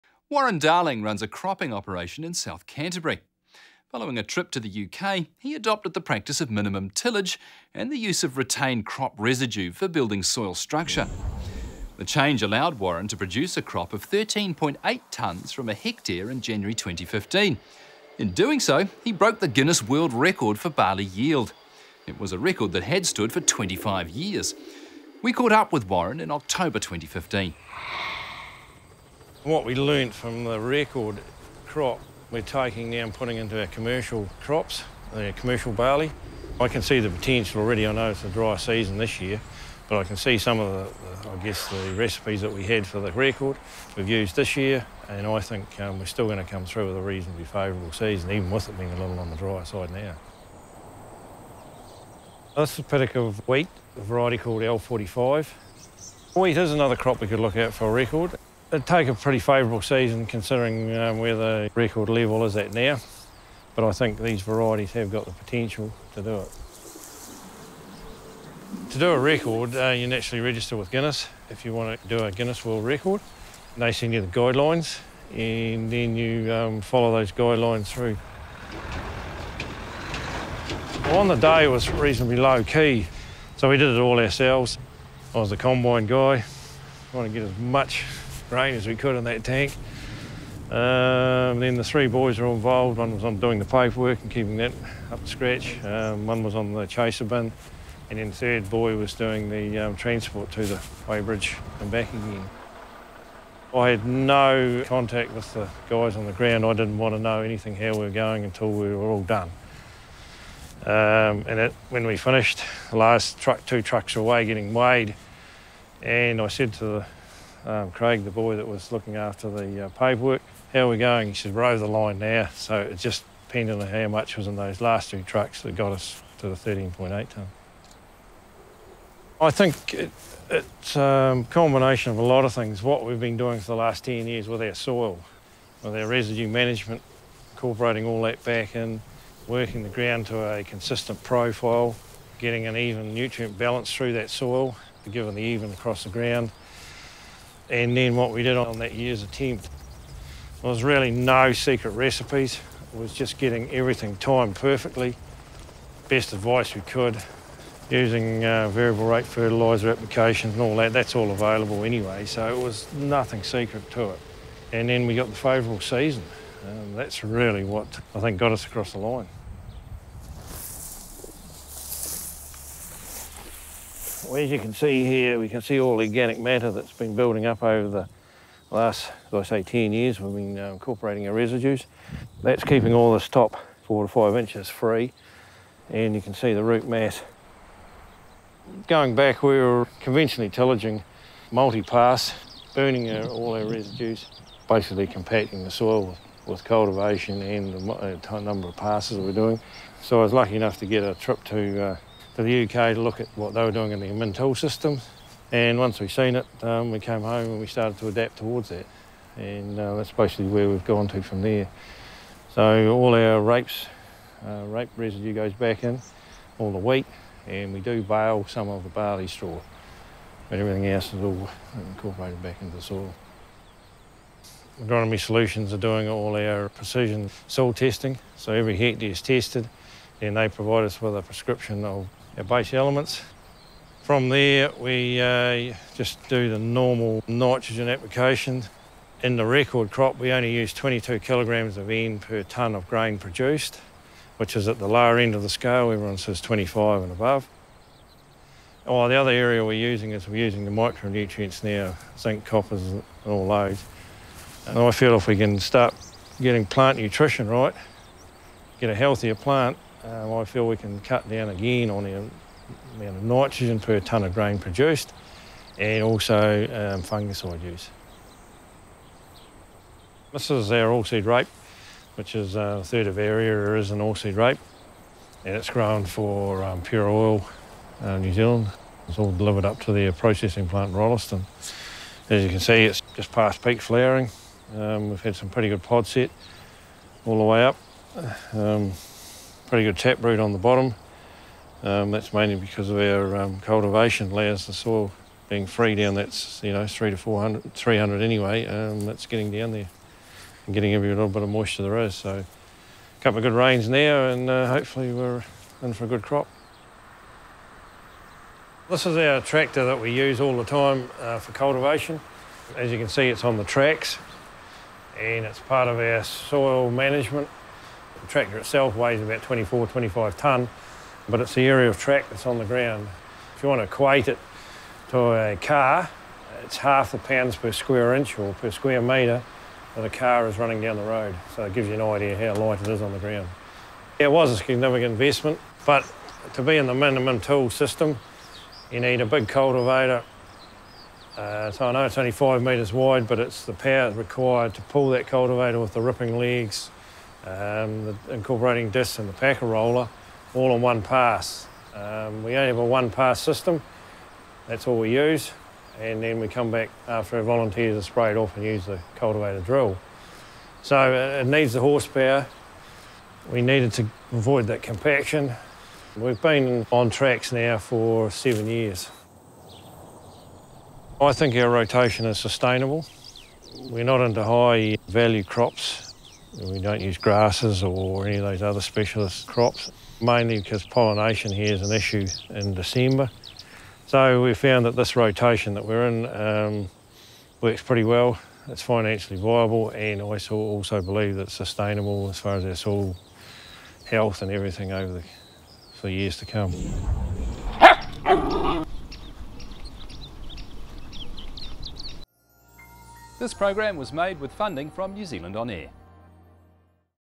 World Record Barley Yield 2015
Minimum tillage and a focus on soil fertility and nutrient loss reduction promotes big yields